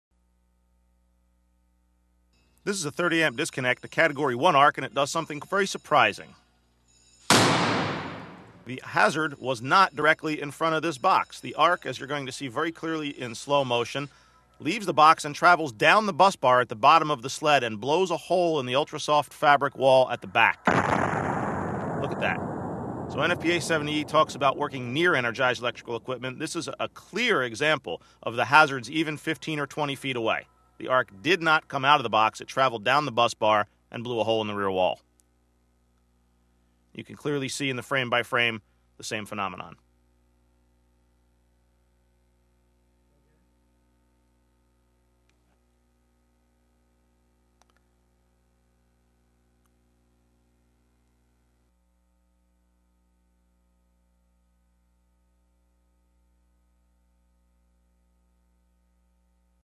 Westex - Live Arc Flash - Top 10 #9
4.0 cal/cm2
480V / 30Amp
Side Angle
No Manikin
Arc travels down the bus sideways and blows a hole in the back wall 10+ ft away.